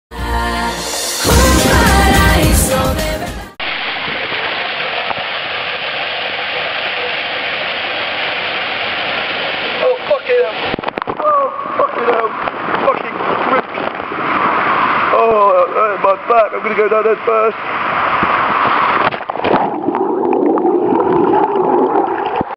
Space Bowl Slide OnRide POV - Kos Lido Waterpark GREECE
A full OnRide look at the Spacebowl slide at Lido Waterpark on the world class party island of Kos, Greece.